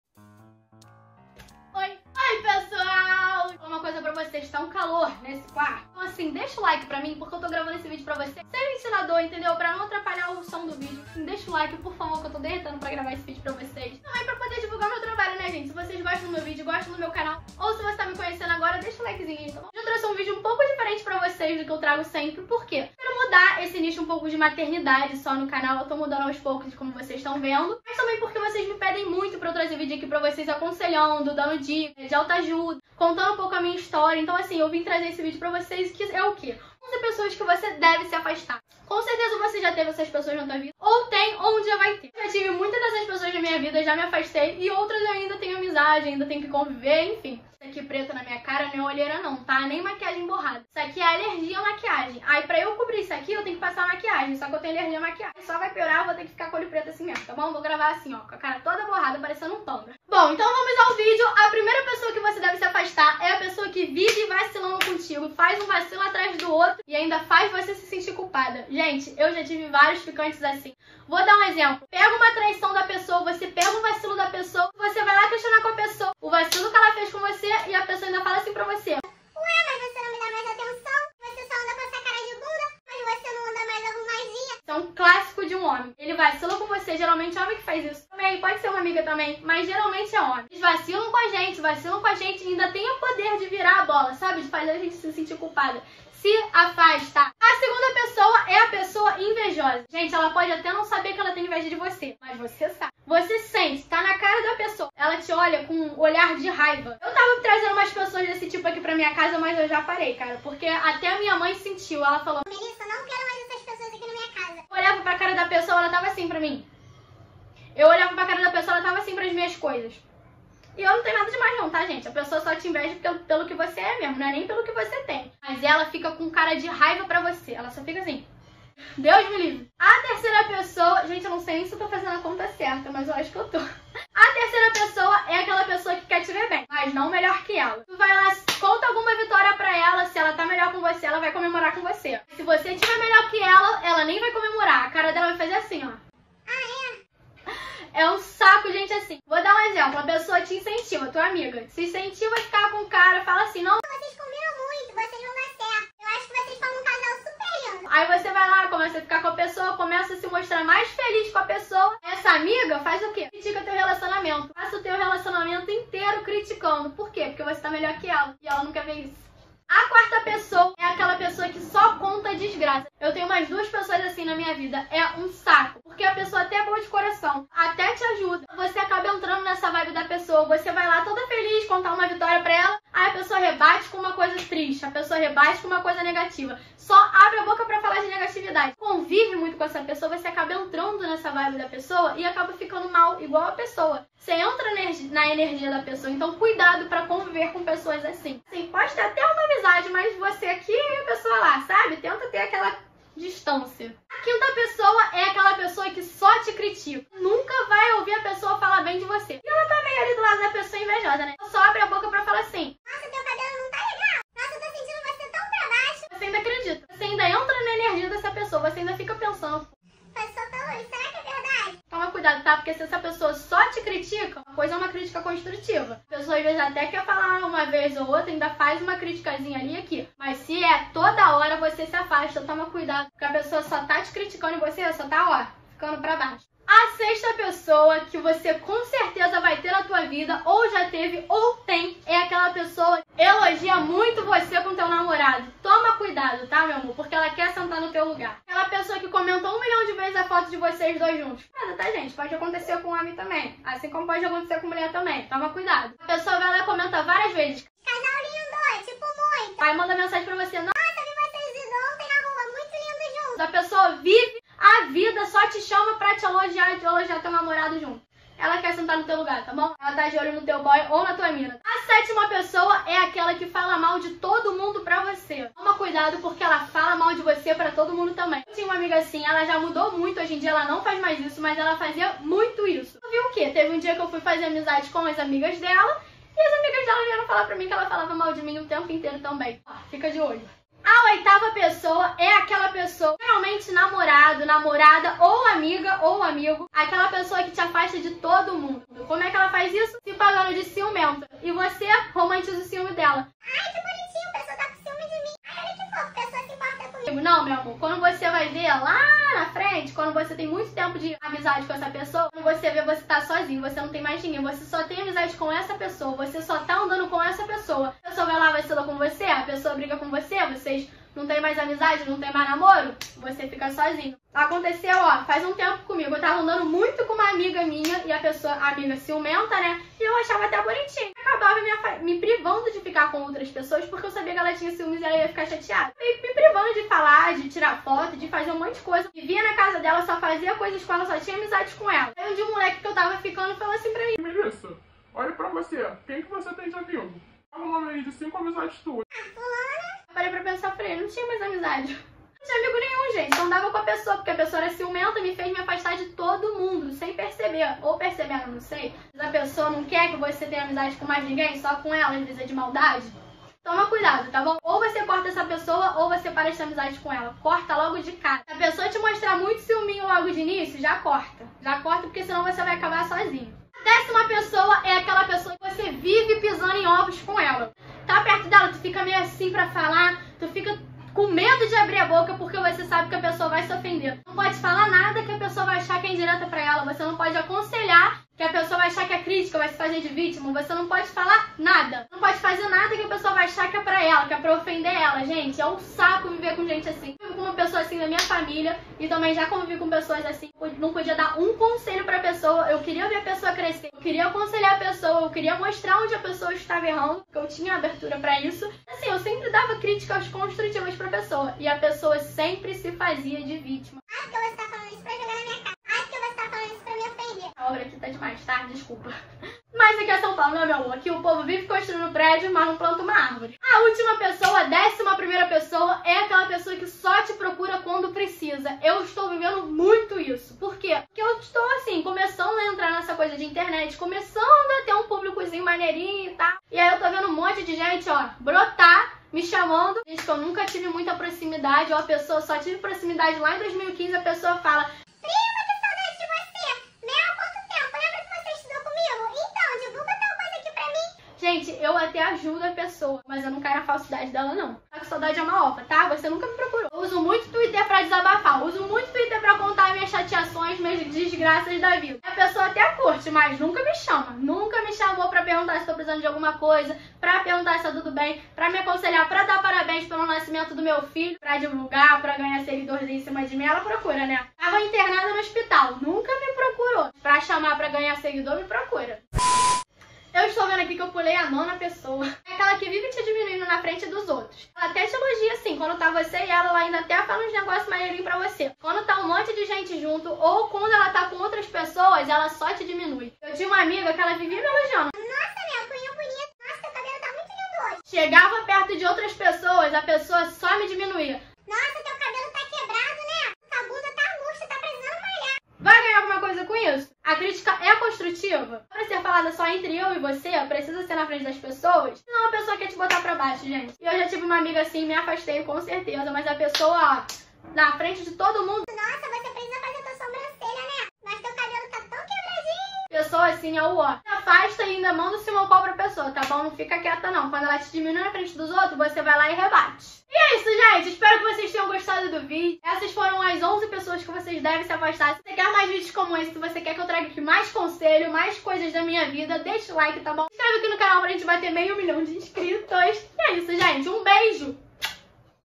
CRÍTICA, INVEJA, NEGATIVIDADE- 11 tipos de pessoas pra você se afastar
Amei trazer esse vídeo pra vocês! Espero que tenham gostado também!!!